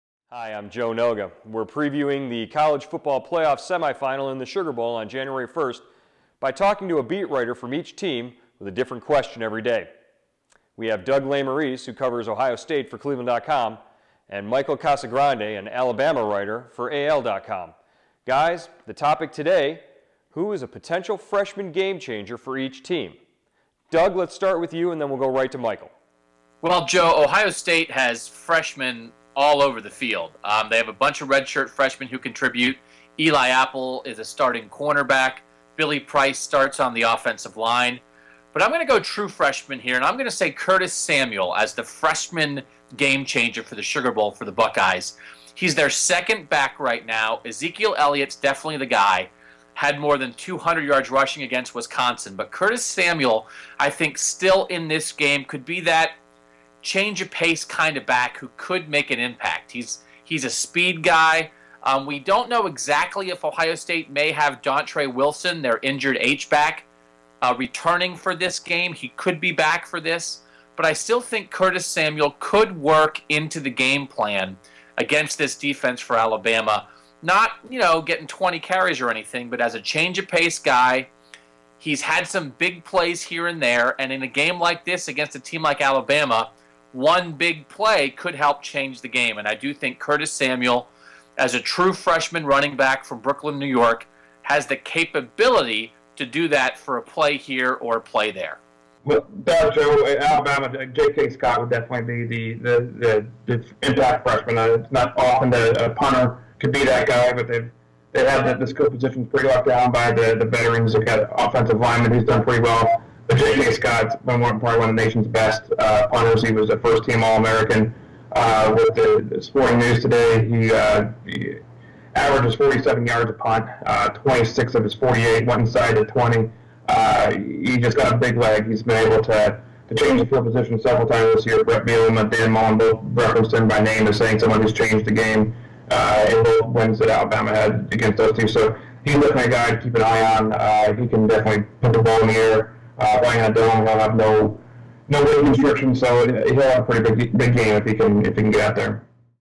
Ohio State vs. Alabama freshman game changers
Writers from cleveland.com and AL.com talk about two players who could be freshman game changers for Ohio State and Alabama in the Sugar Bowl.